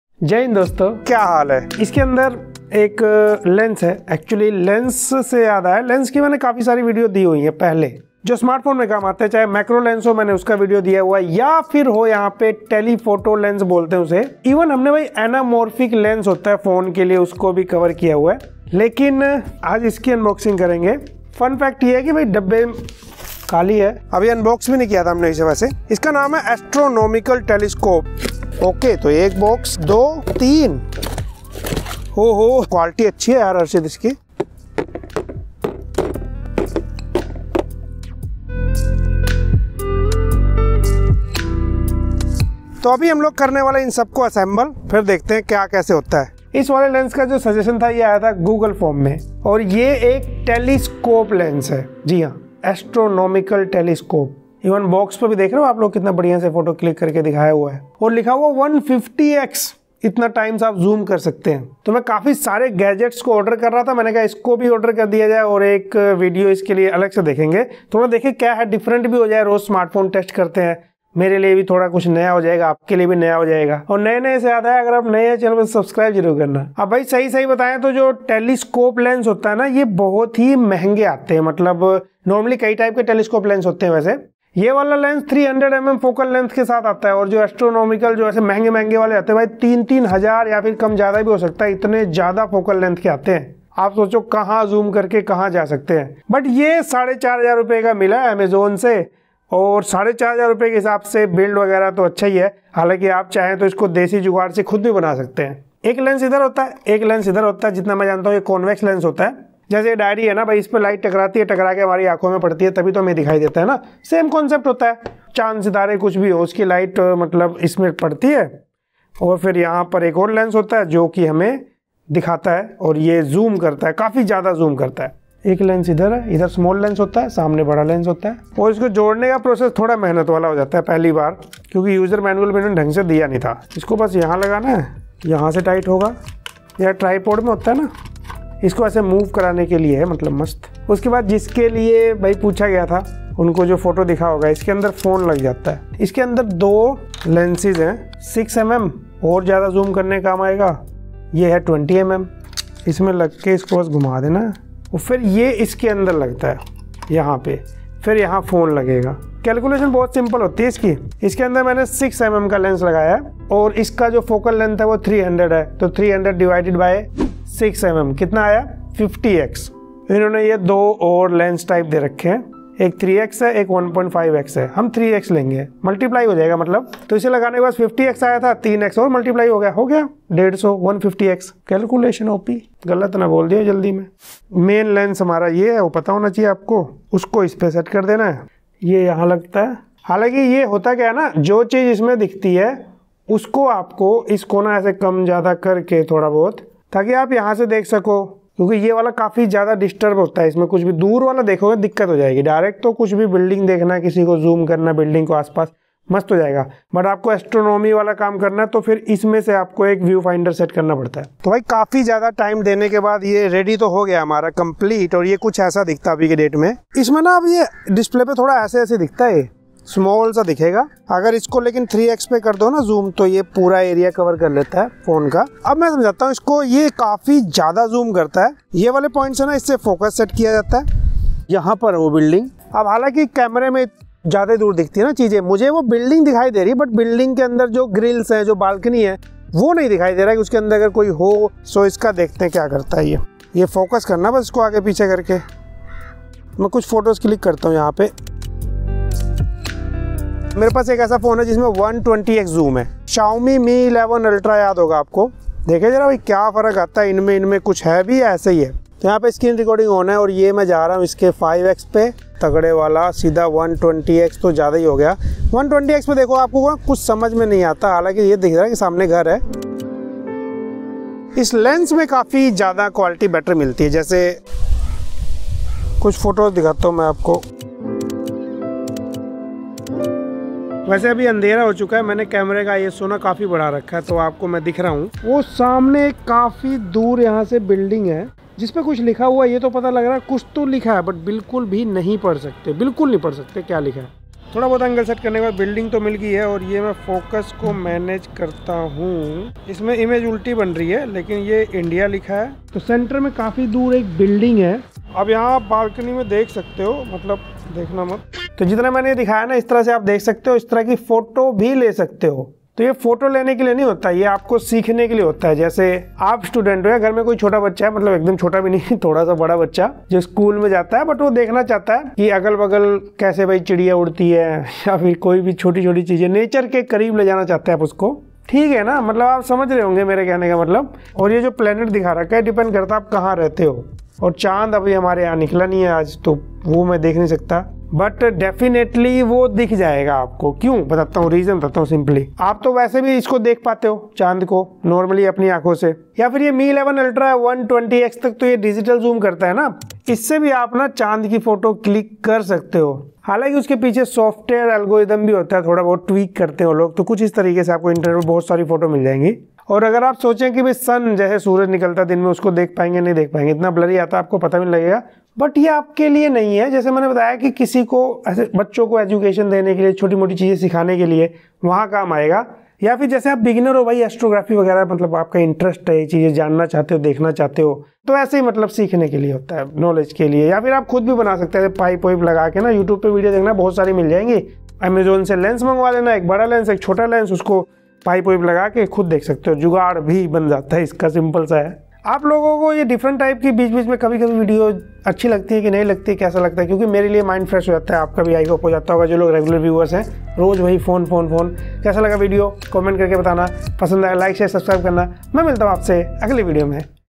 This Telescope Lens comes with 150x Zoom Power !
This is a basic telescope lens for Astronomy. This lens is not for Professional use.
It comes with 150X HD Refractor Telescope lens for zoom and you can use mobile as view finder for this lens.

*This lens is not for all types of users.